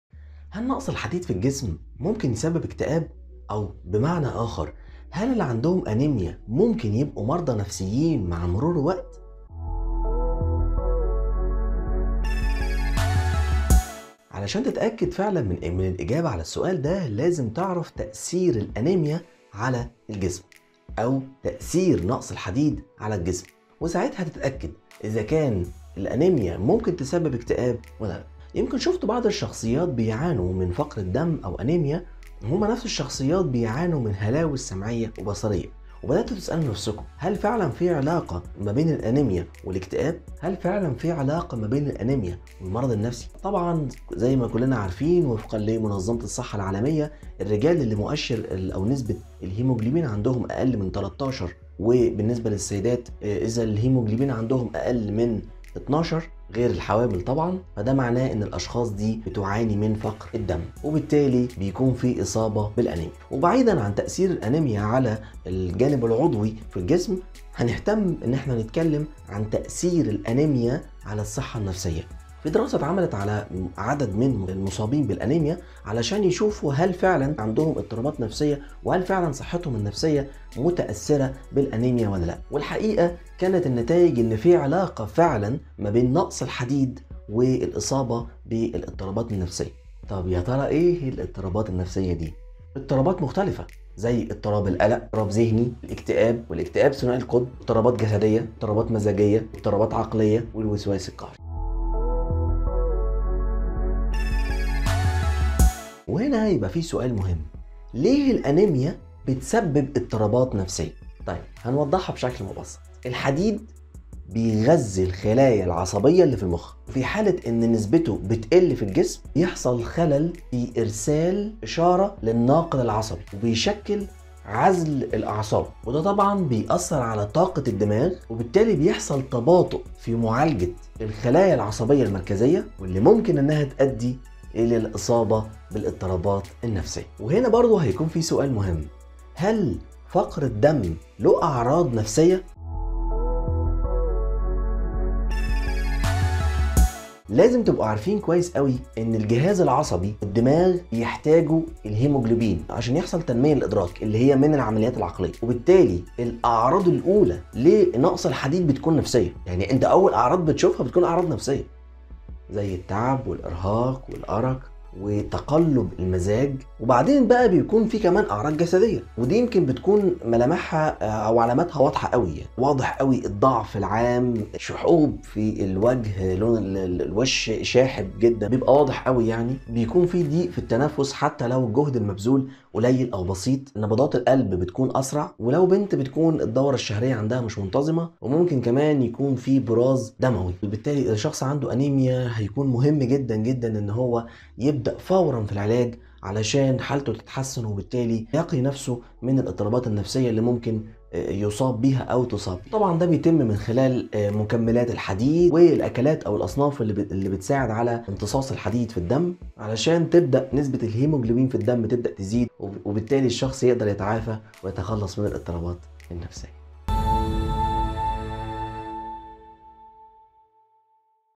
هل نقص الحديد في الجسم يسبب الاكتئاب !؟ | د.محمود رضوان
هل يمكن أن يتسبب نقص الحديد في الجسم بالإصابة بالإضطرابات النفسية  !؟

هل المصابين بالأنيميا أو فقر الدم مُعرضين للإصابة بالإضطرابات النفسية !؟

ما تأثير الأنيميا أو نقص الحديد على الجسم ؟

ما تأثير الأنيميا على الصحة النفسية ؟

هل هناك علاقة بين نقص الحديد والاضطرابات النفسية ؟

لماذا تتسبب الأنيميا بـ الاضطرابات النفسية ؟

هل فقر الدم له أعراض نفسية ؟

أهم الأعراض النفسية والجسدية للرجال والنساء

أهم خطوة للعلاج حتى يمكن للشخص التعاف

هذا الفيديو جزء من البرنامج للاستشارى النفسي د.محمود رضوان

لمزيد من المعلومات يمكنك زيارة الموقع

نتشرف بتفاعلك معنا على صفحات التواصل الاجتماعى